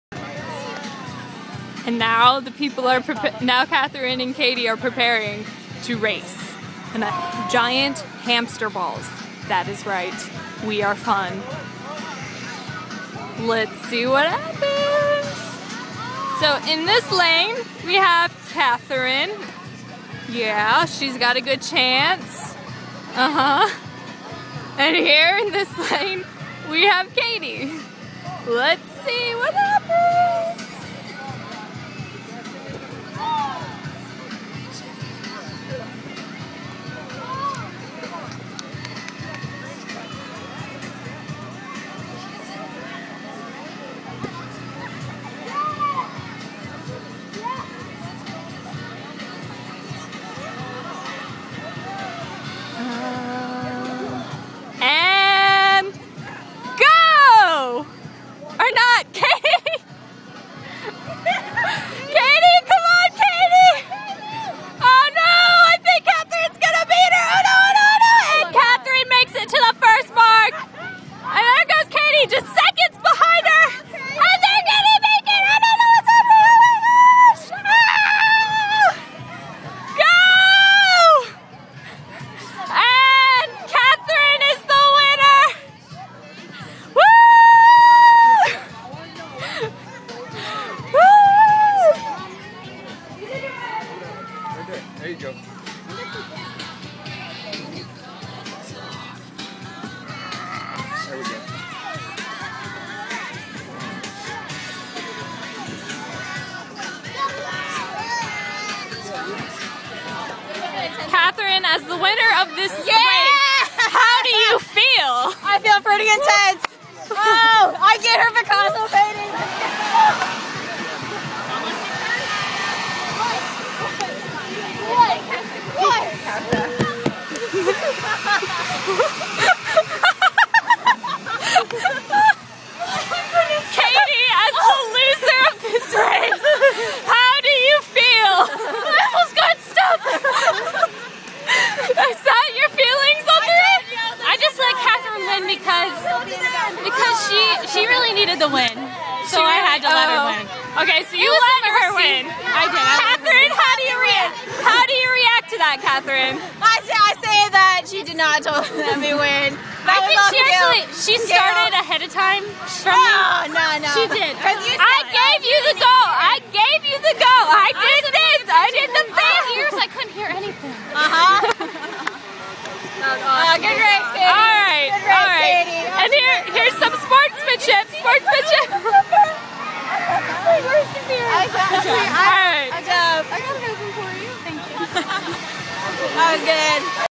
Human Sphere Race
Katie Vs Catherine for the ultimate "Picasso Piece" and bragging rights ;D
Narrated by Sarah